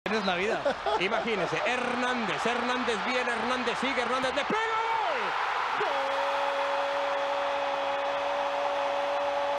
Gol del año: Chicharito vs Ecuador.
Incondicionales, el 2015 fue un año de grandes goles y necesitamos su ayuda para decidir el mejor. ¿Será este derechazo de Chicharito? ¡Participen!